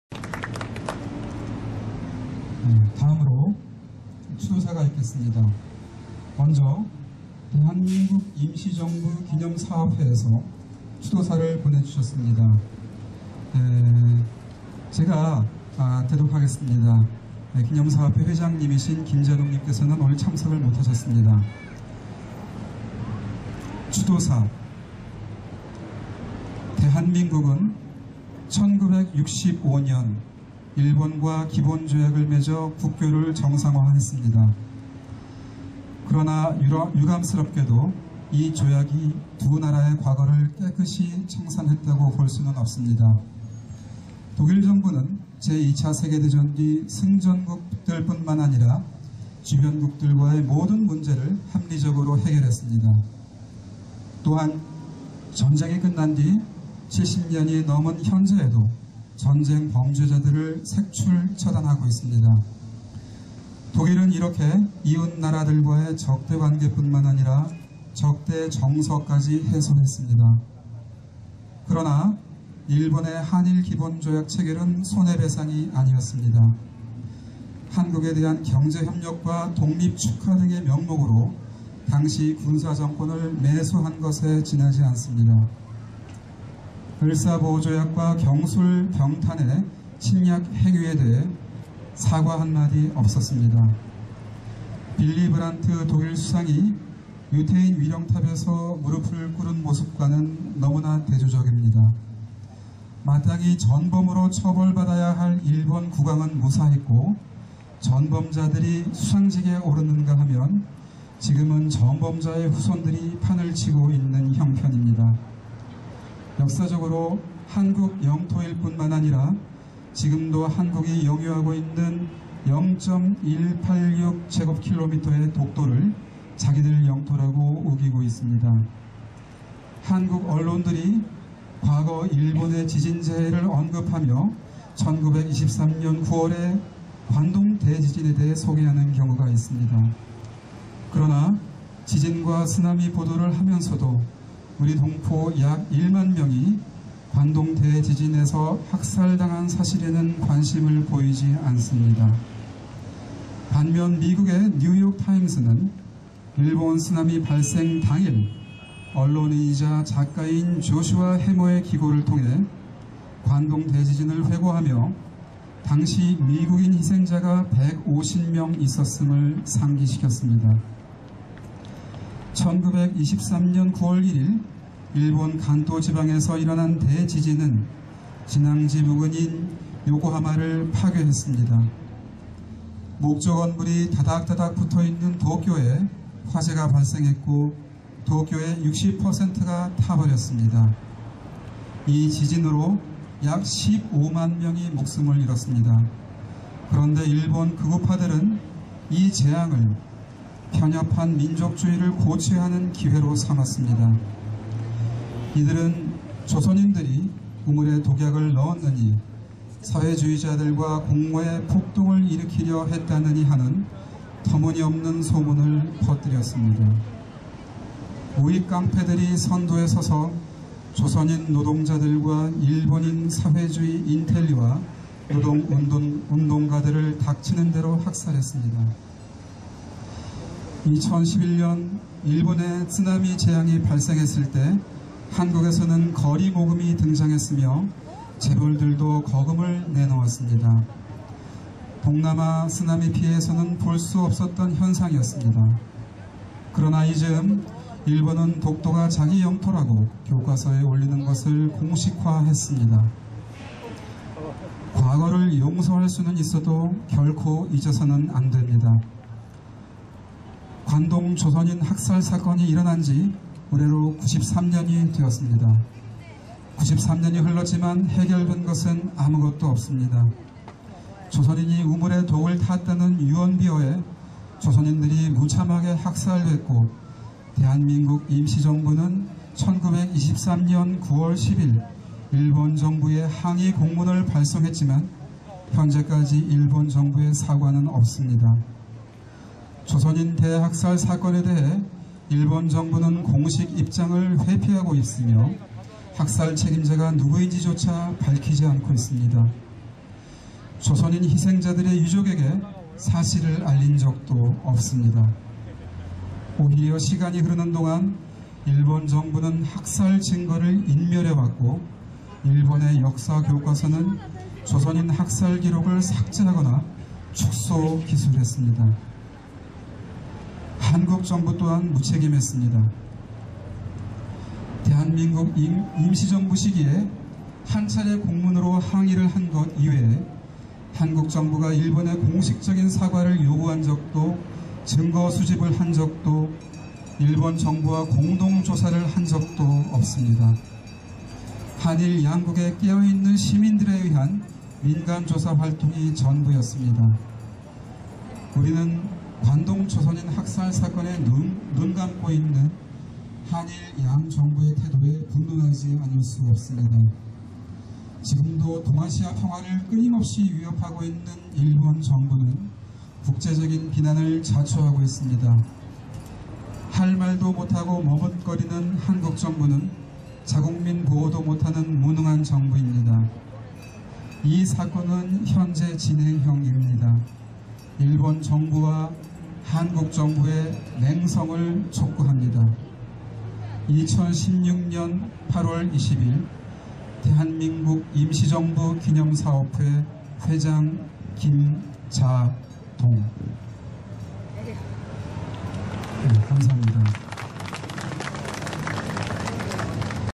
추도사 대한민국 임시정부 기념사업회, 2016 관동대학살희생동포위령
1923년 관동대지진 때 학살당한 한인들의 추도식
2016년 8월 20일 광화문 광장 북측

1923년 9월 1일 일본 관동지방에서 발생한 대지진 직후에
6천명이 넘는 한인들이 학살당한 사실이 있다
대지진이 발생한 도쿄와 그 주변 지역에서
조선인이 방화하고, 우물에 독을 던지고 폭동을 일으켰다는
근거 없는 유언비어가 확산되었고,
이를 광신한 일본의 군경 및 민간 자경단이
한인들을 학살한 사건이다.

이 1923년의 한인 대학살 사건은
전시가 아닌 평시에 일어난 야만적인 사건이었다.
그러나 일본 정부는 오늘날까지 그 한인 대학살 사건에 대해
마치 없었던 일처럼 일체 함구하고 있다
한국 정부도 지금까지 이에 대해 진상규명을 하거나
일본 정부에 항의를 한 적이 없다.